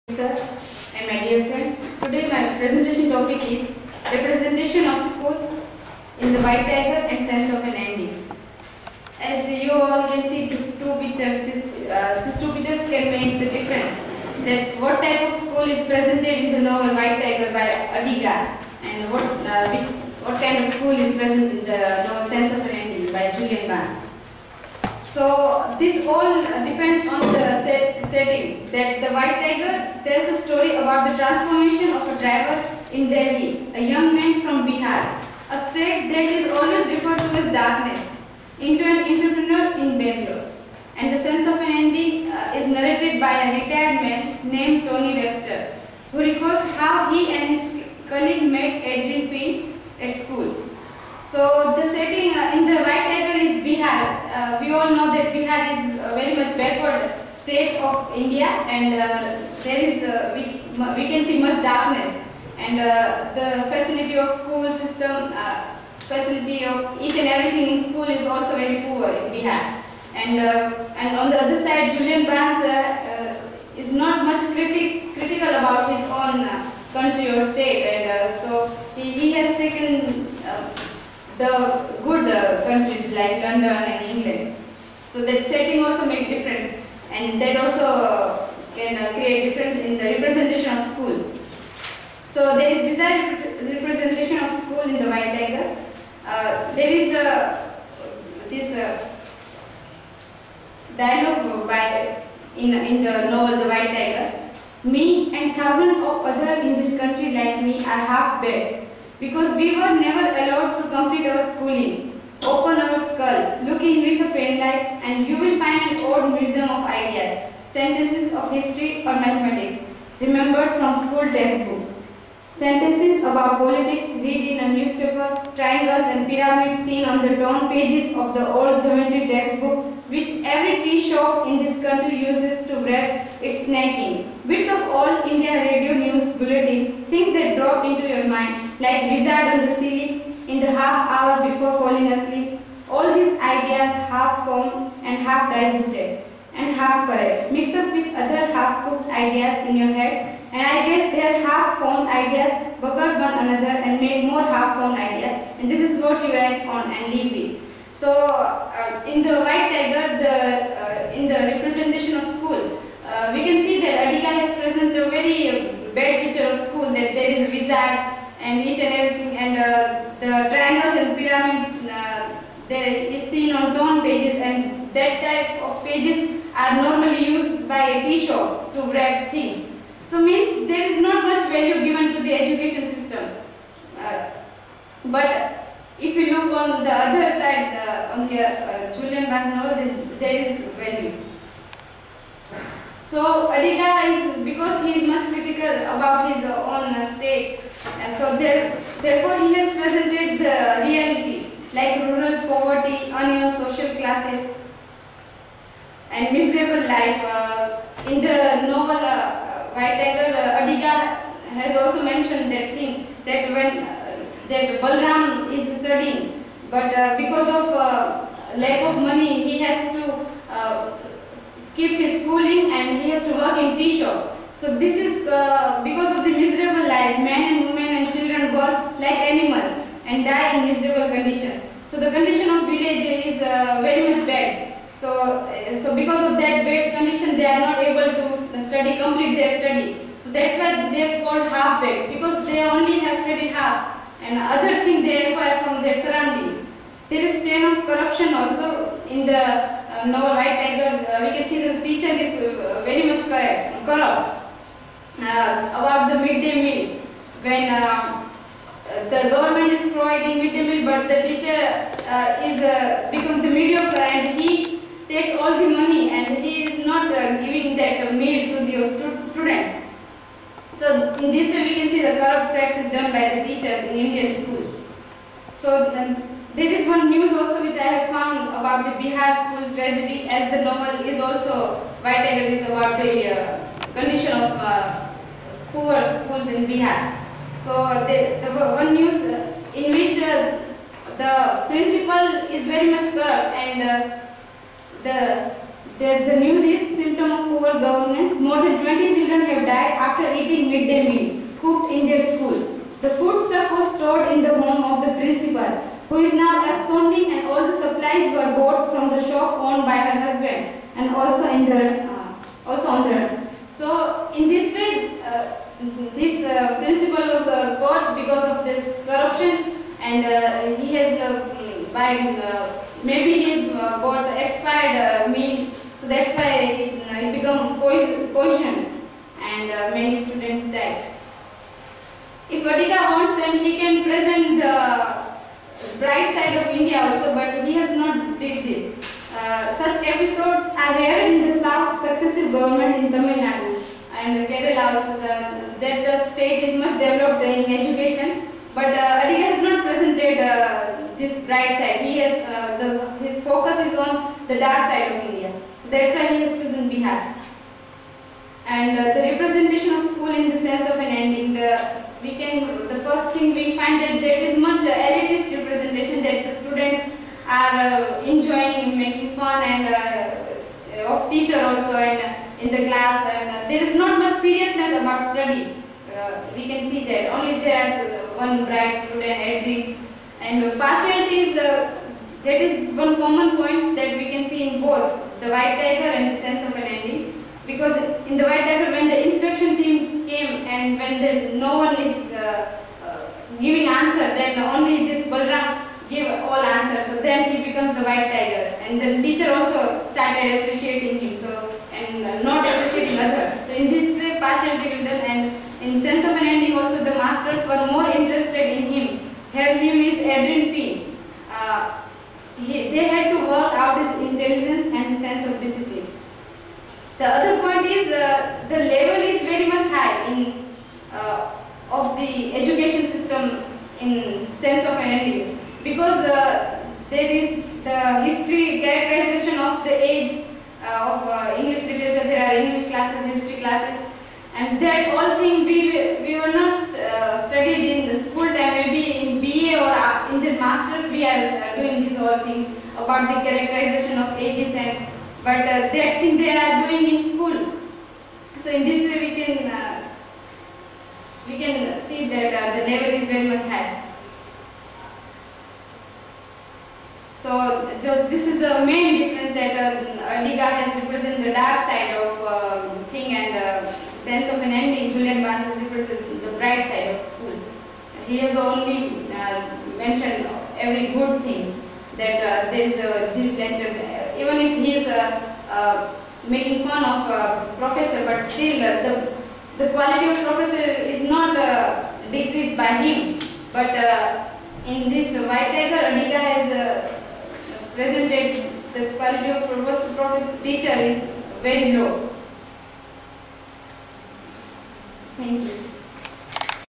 Representation of School in "The White Tiger" and "Sense of an Ending"
This academic presentation on New Literature